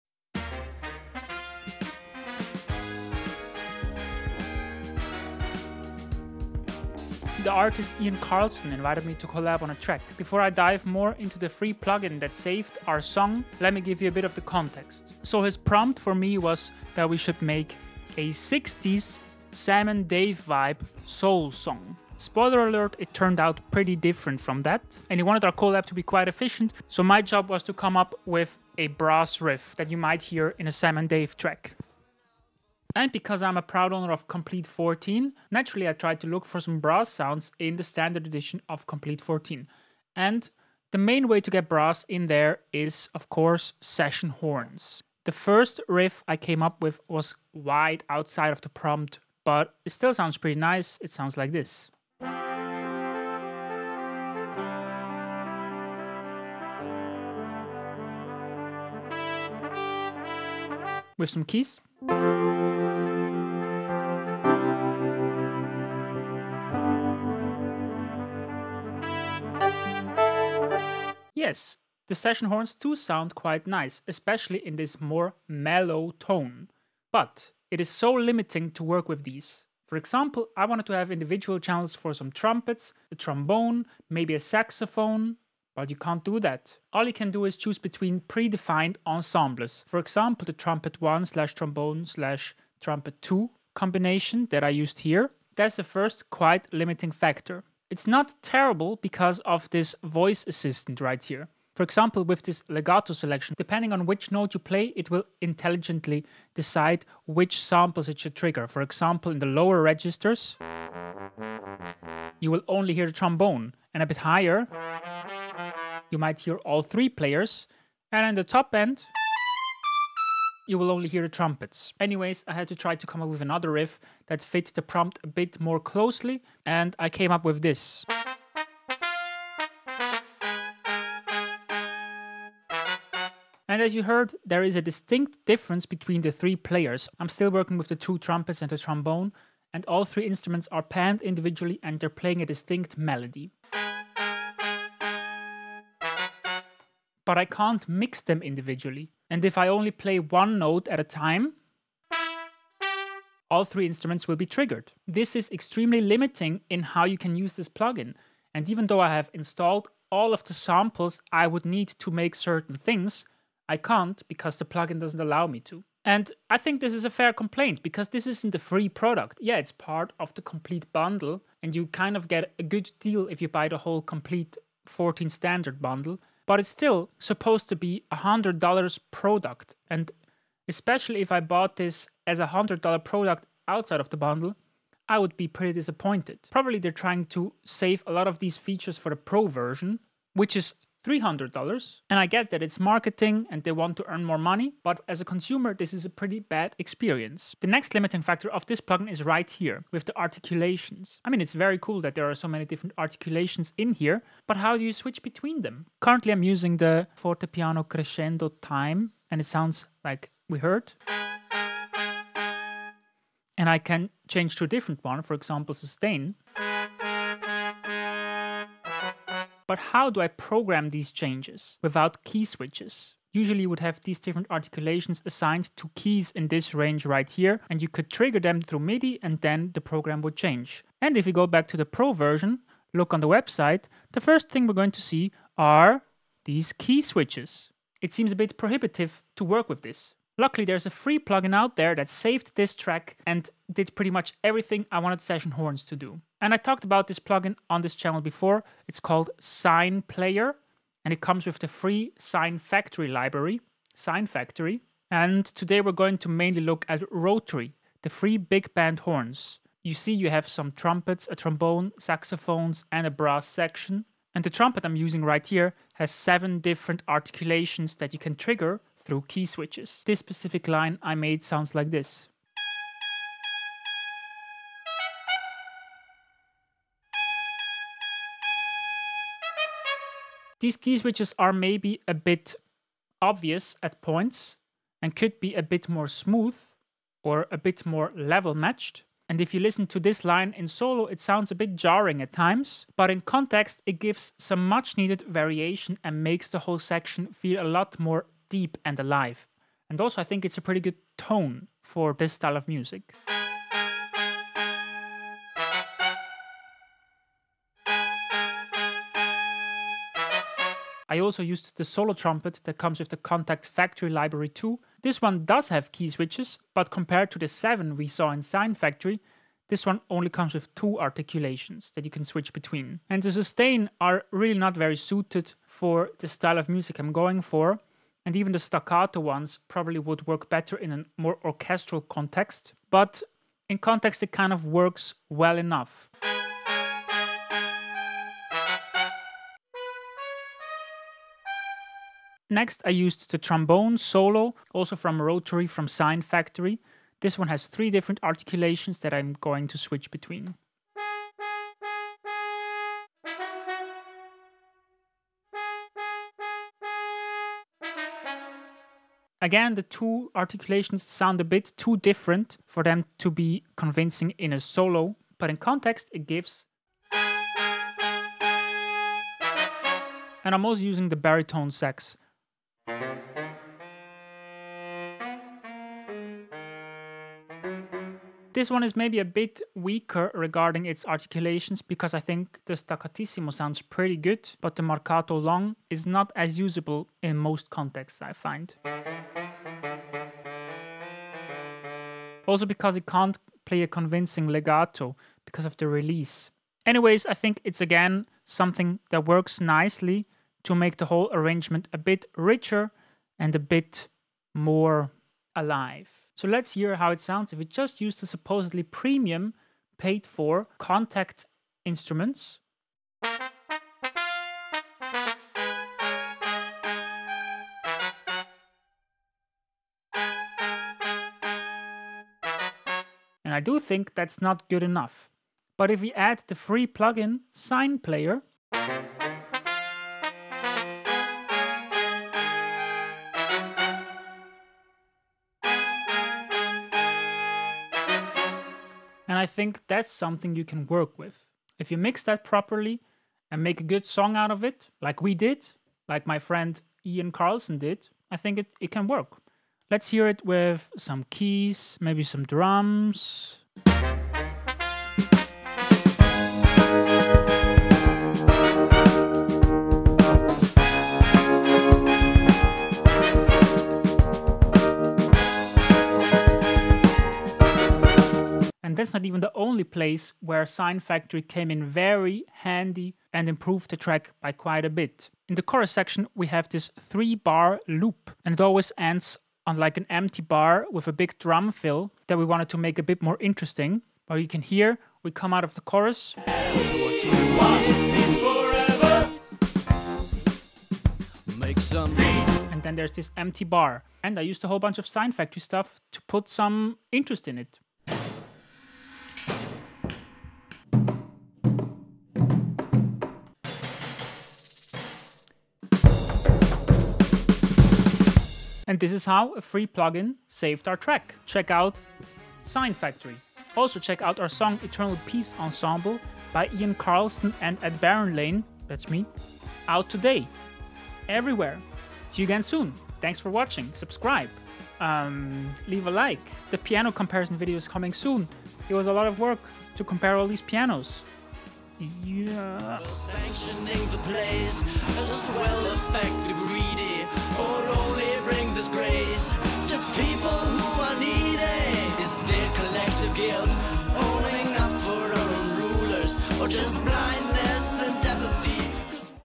This FREE Plugin saved our new Song!
Komplete 14 standard has some useful sounds. But is Session Horns - the seemingly most powerful Brass Library - one of those? Or ist it quite a bit too limited in functionality? I was working on a new track and found a more powerfull Brass collection in the Free Plugin and Sample Library collection called "Sine Factory" by Orchestral Tools. 'Rotary' are quite usable Free Big Band Horns.

Or just send a superthanks my way! :D

00:00 - Some context
00:32 - Brass Sounds in Komplete 14 standard
01:00 - first attempt
01:20 - One Big Problem with Session Horns
02:03 - second attempt
02:15 - Session Horn's Voicing Assistant
02:47 - Rant 1
03:15 - Another big problem with Session Horns
04:00 - The FREE PLUGIN that saved our song
04:40 - KEY SWITCHES in Sine Factory Rotary
05:10 - Kontakt Factory Library 2
05:42 - more Rotary Brass
06:09 - Rotary Baritone Sax
06:46 - Comparison "Kontakt" vs "Kontakt + SINE"
07:34 - Song Preview
07:48 - Great Percussion from Sine Factory Dynamo
08:30 - Eternal Peace Ensemble